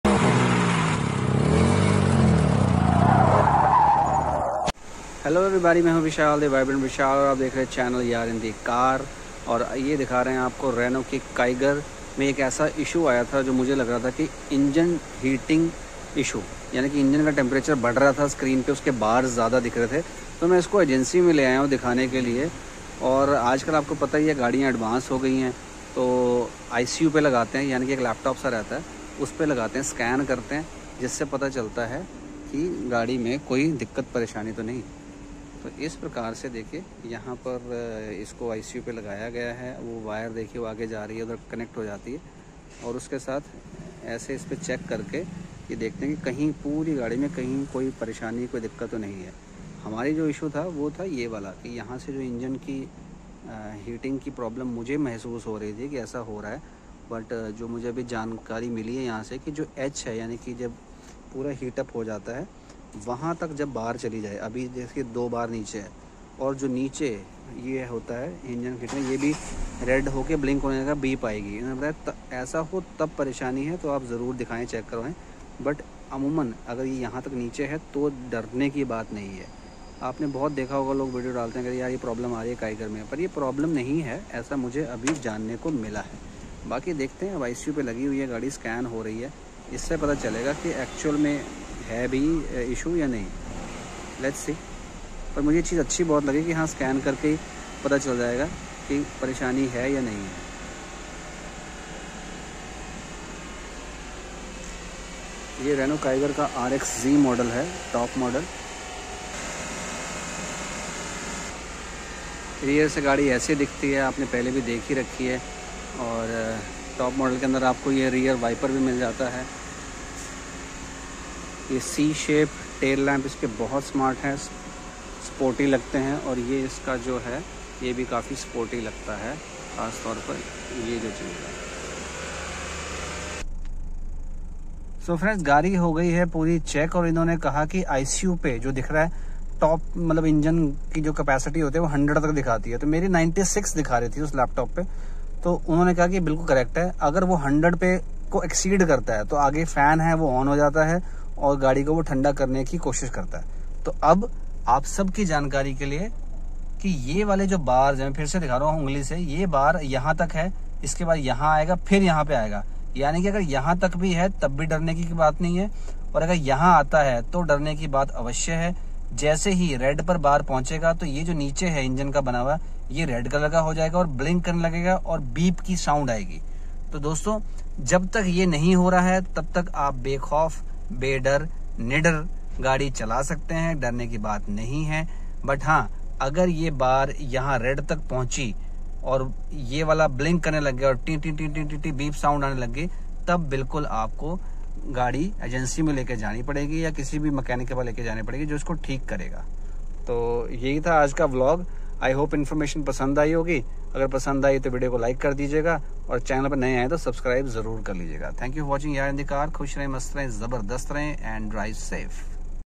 Renault Kiger Engine temperature🌡️Problem Clarified/Resolved@yaarinthecar4955
In this video you will see that how much should be Renault Kiger engine temperature bars level should be. and when it's alarming and you need to visit mechanic or service station.